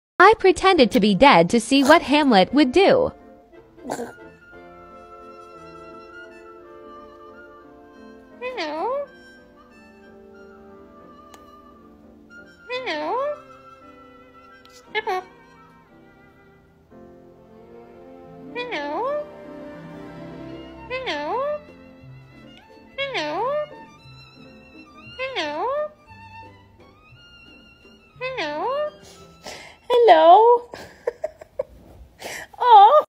I pretended to be dead to see what my bird would do
Via @chantyb97, TikTok